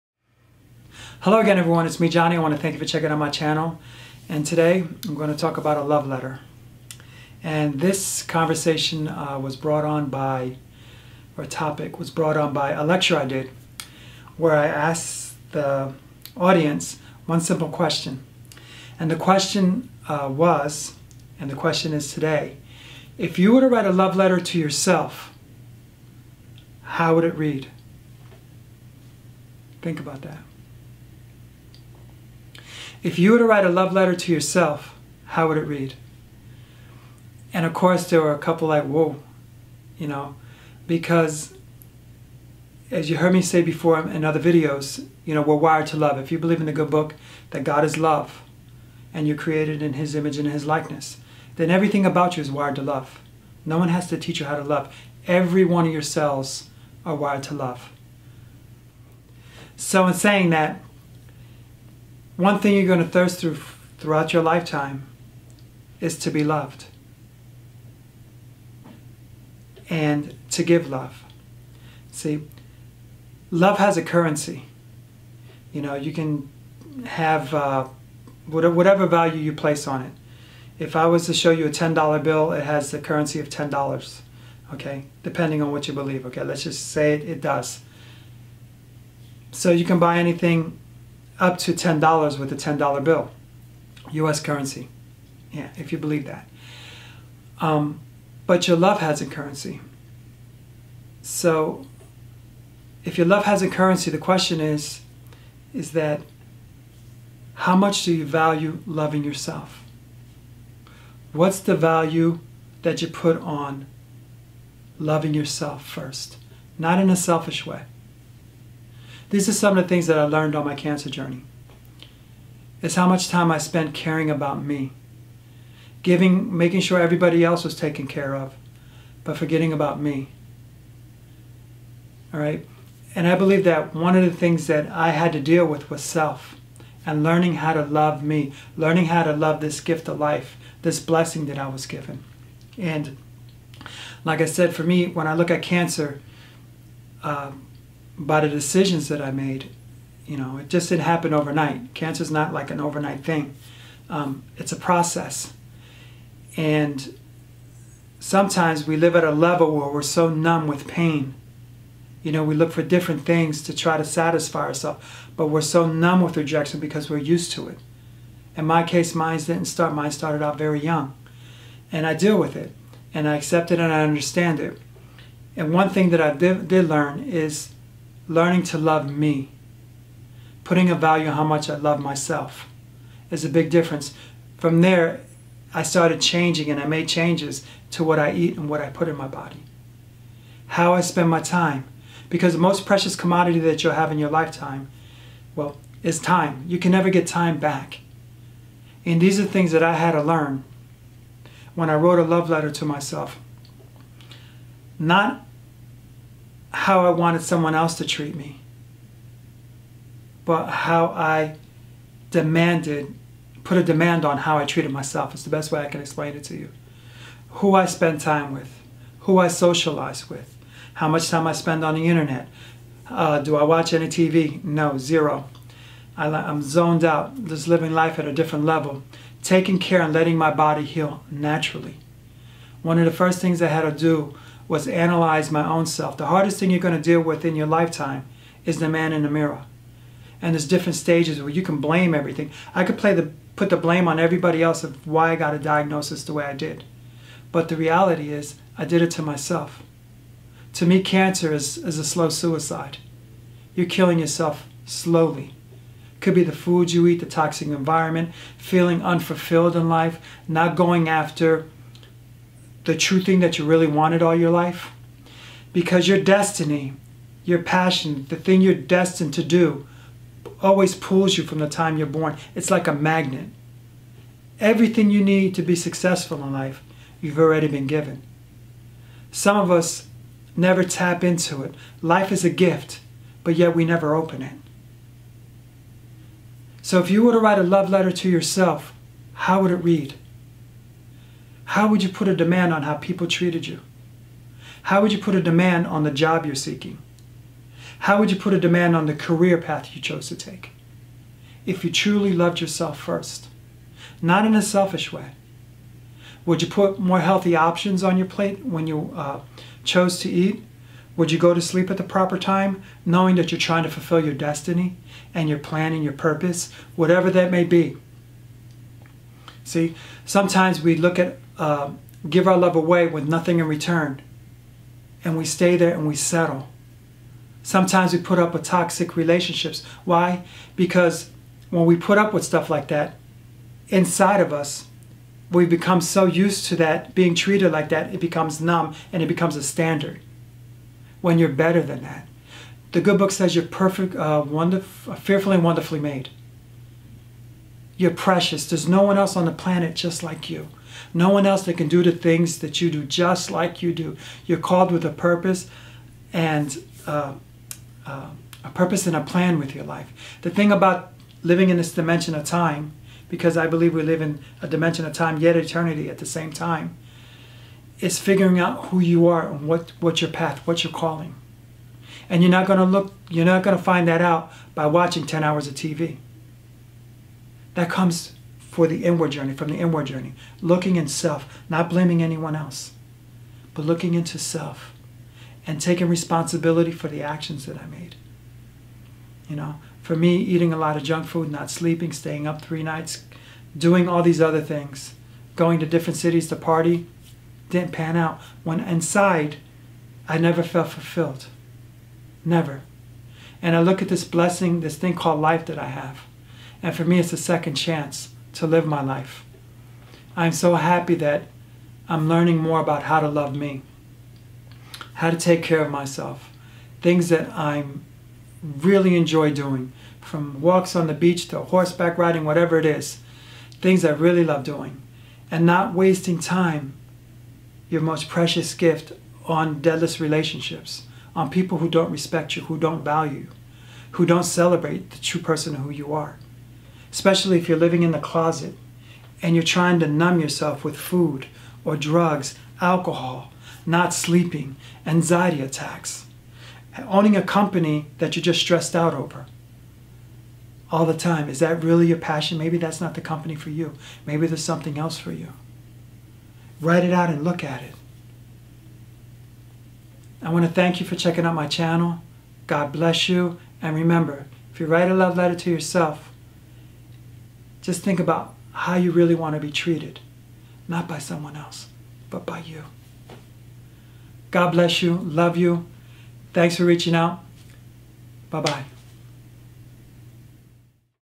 A Love Letter
A Love Letter is about the value one puts on loving themselves, and setting boundaries on how we let people treat us.